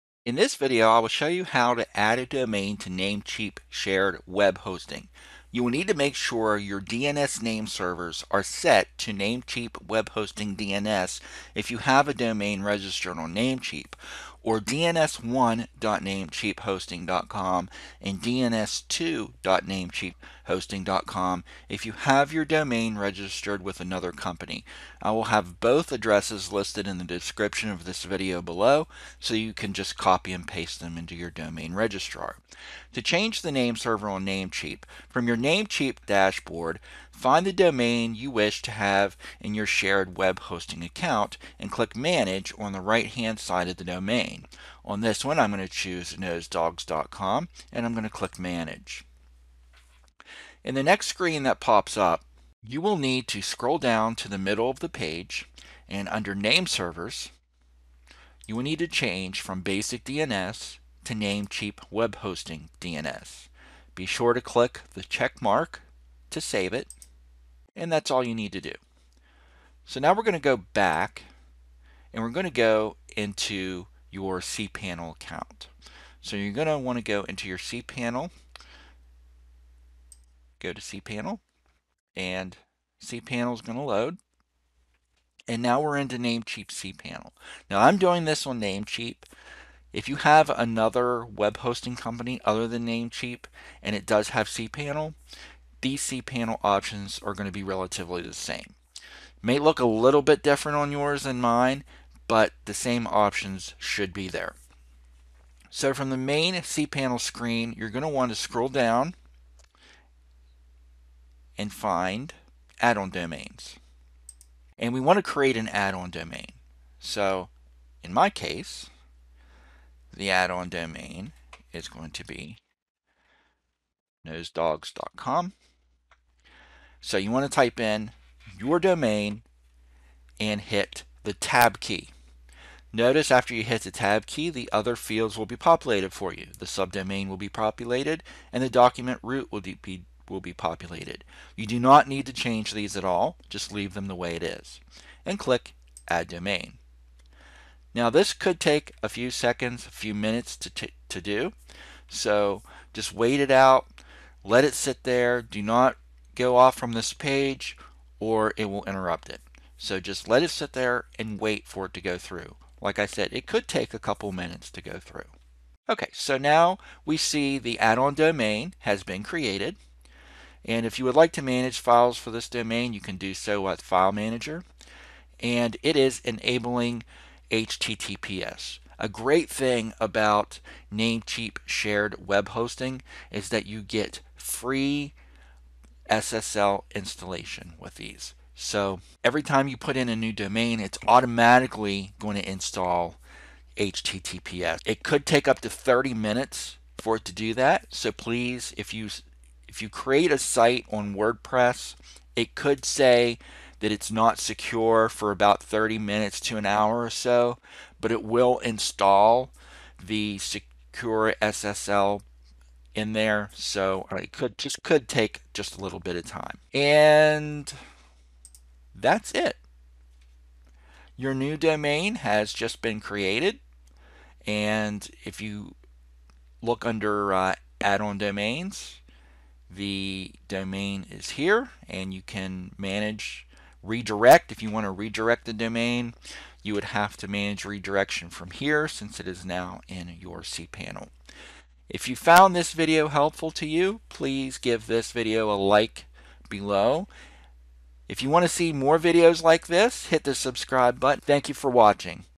How To Add A Domain To Namecheap Shared Web Hosting | Namecheap Addon Domain
In this video I will show you how to add a domain to Namecheap shared web hosting. We will be doing this in your Namecheap cPanel dashboard.

DNS Nameservers for Namecheap Shared Web Hosting:
Namecheap: Namecheap Web Hosting DNS